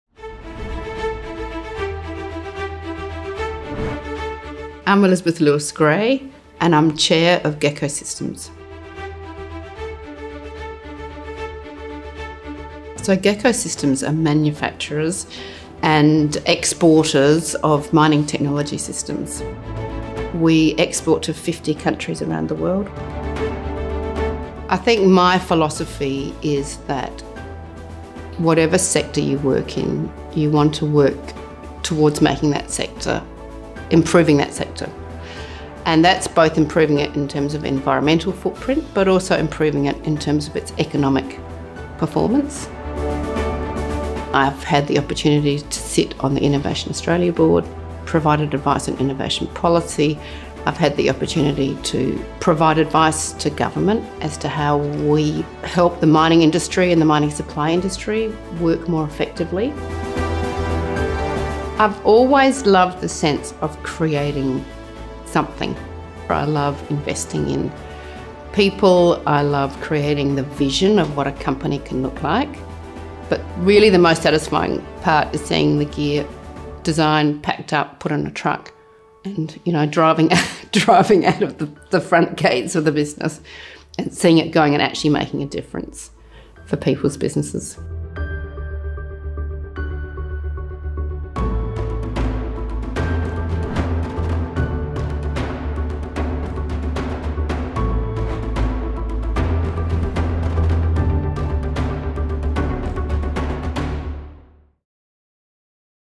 VMHOF Awards 2023 – Winner in Honour Roll for Service to Industry category
The winner of the 2023 Victorian Manufacturing Hall of Fame Award for Honour Roll for Service to Industry category was Elizabeth Lewis-Gray.